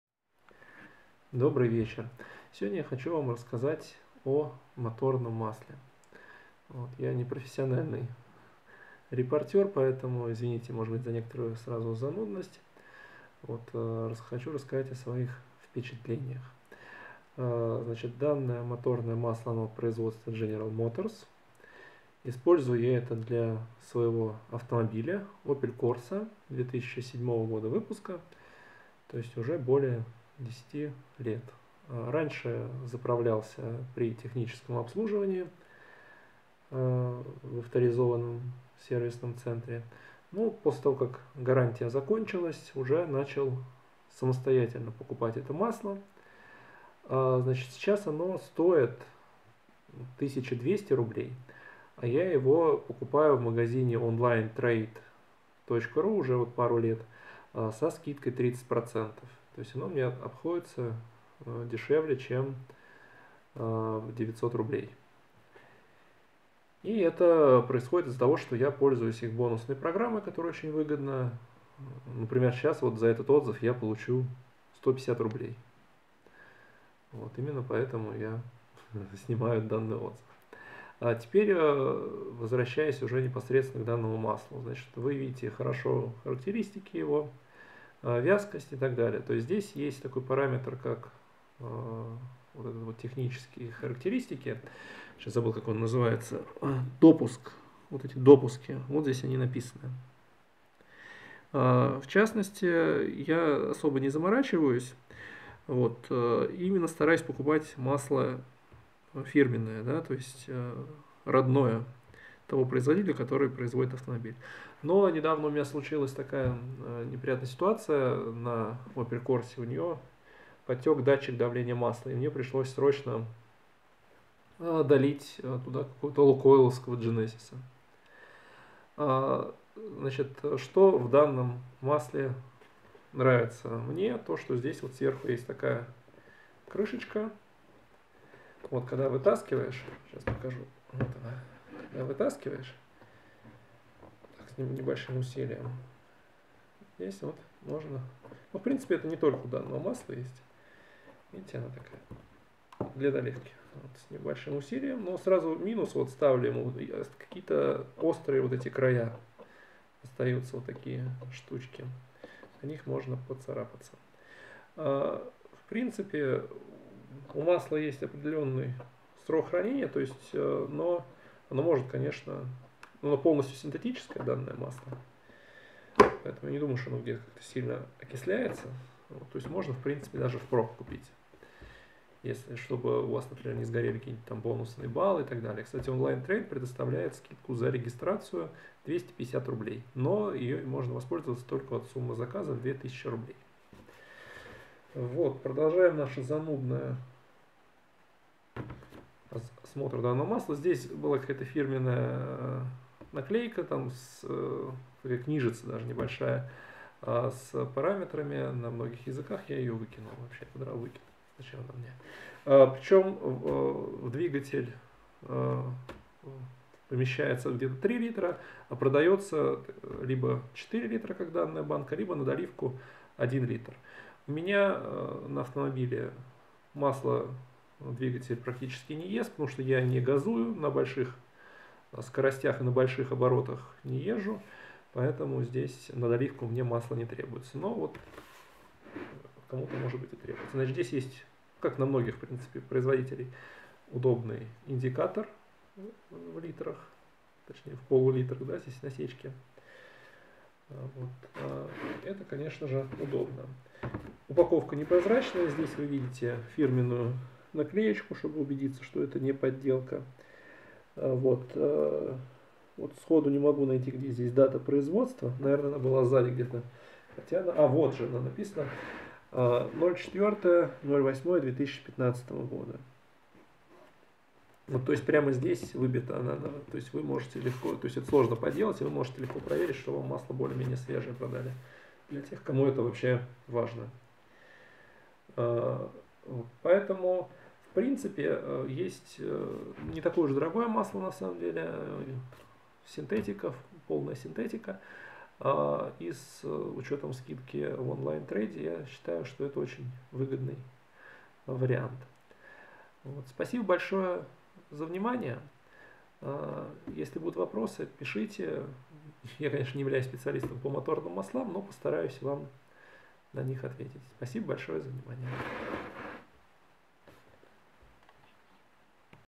Отзыв о моторном масле General Motors Dexos2 Longlife 5W30 4 л
Обзор моторного масла General Motors motor oil Dexos 2 Longlife 5W-30 от обычного покупателя. GM производит качественное полностью синтетическое масло, в частности, для opel. С подделкой не сталкивался.